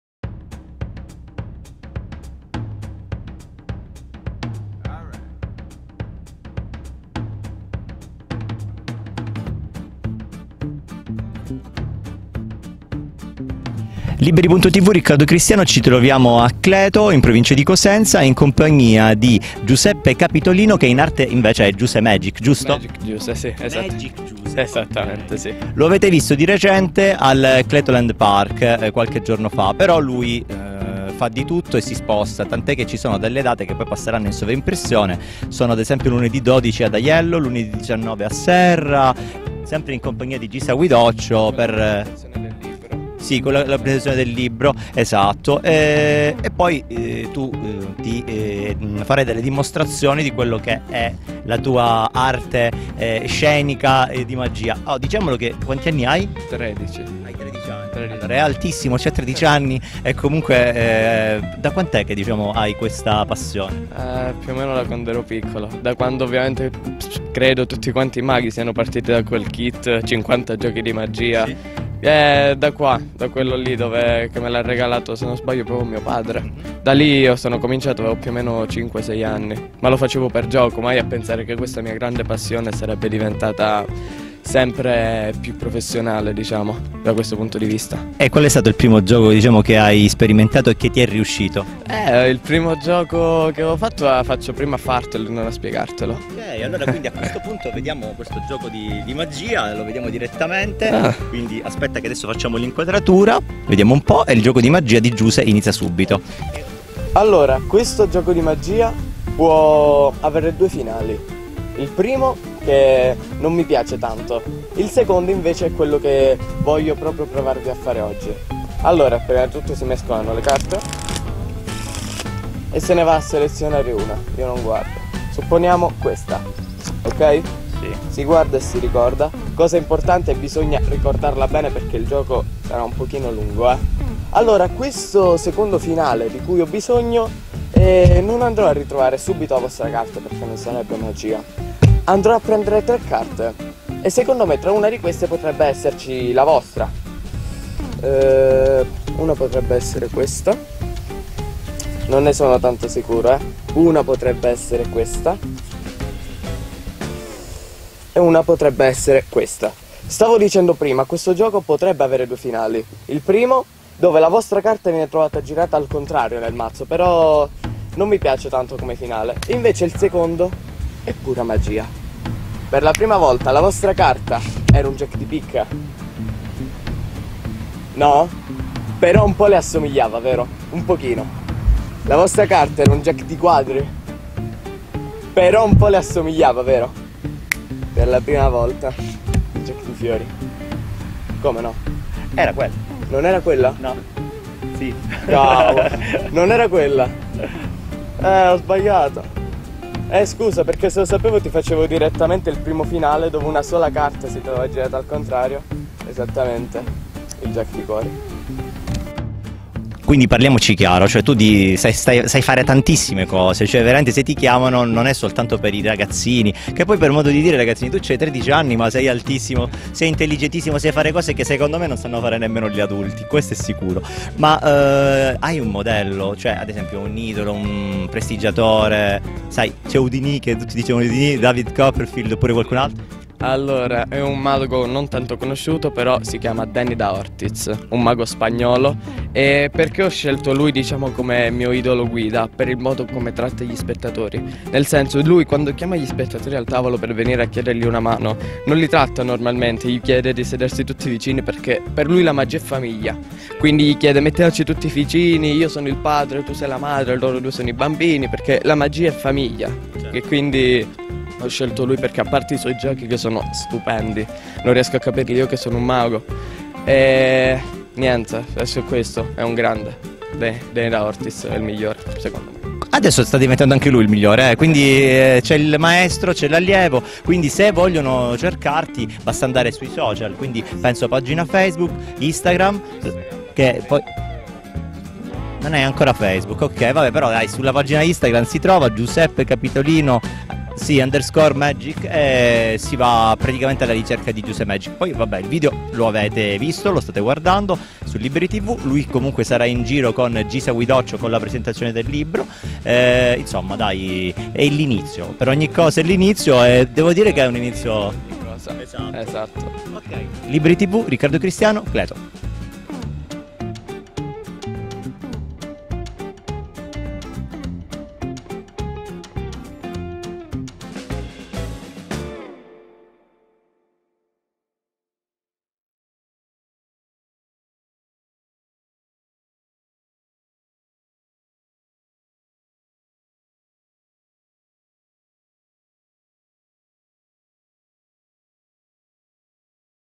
Intervista a Giuseppe Capitolino in arte Magic Giuse - Prestigiatore
Registrazione effettuata l'11 agosto 2019
Intervista di Riccardo Cristiano a Giuseppe Capitolino in arte Magic Giuse - Prestigiatore